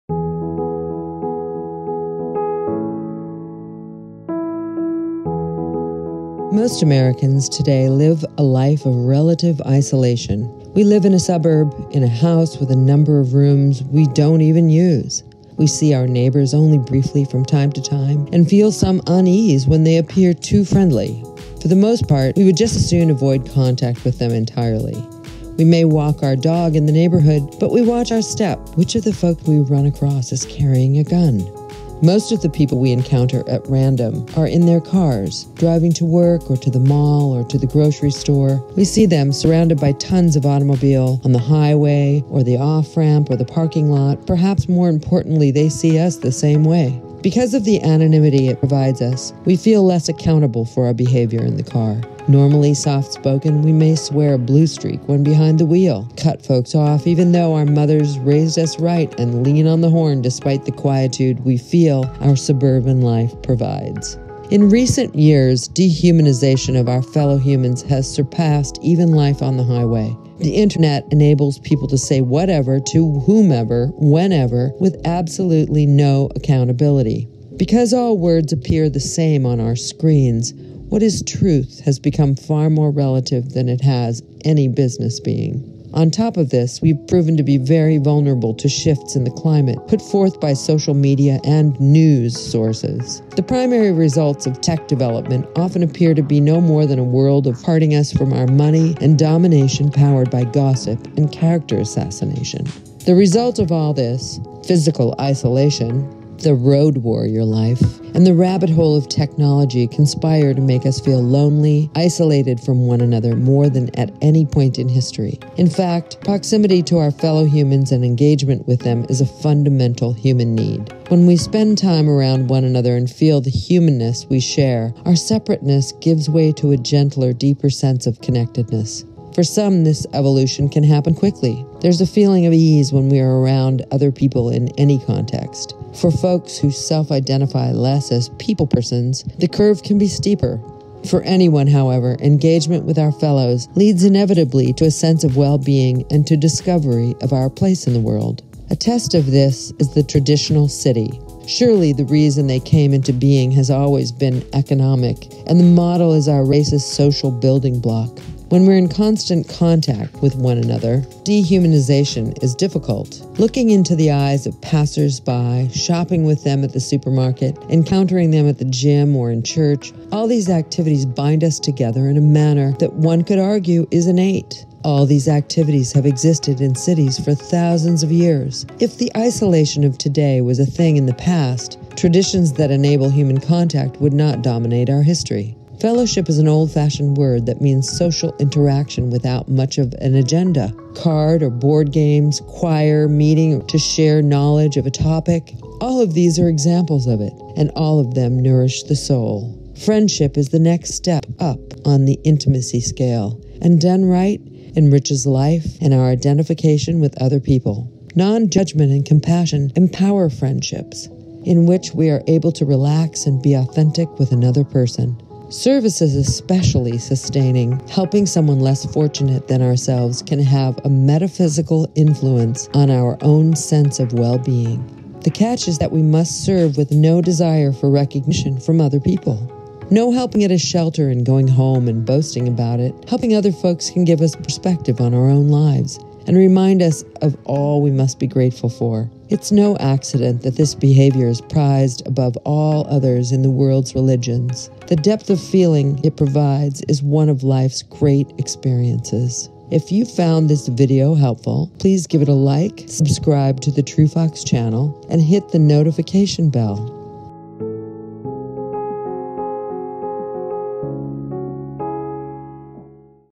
Fellowship Friendship and Service [Wellness Episode 2]
Fellowship, friendship and service describe the different forms of human relationship we can create as an antidote to the isolation we all experience in contemporary life.

This episode talks about the the importance of our interpersonal relationships outside the home and thier role in our feelings of connectedness and wellbeing. It views fellowship, friendship, and service as vital human experiences and relief from the mounting feelings of isolation that plague the human race today.

The suburb, our cars, our increasingly digitized and dehumanaizing work experience, and social media all conspire to cut us off from one another. The separateness that results is not innately human: history shows this.

Reconnection as we describe it ranges from the most offhand (fellowship) to the more committed (freindship). Service as we describe it- the selfless giving to our fellow humans without any expectation - is the strongest creator of interconnectedness , which explains its prominence in all the worlds religions.

Please subsribe to the truefox.Channel!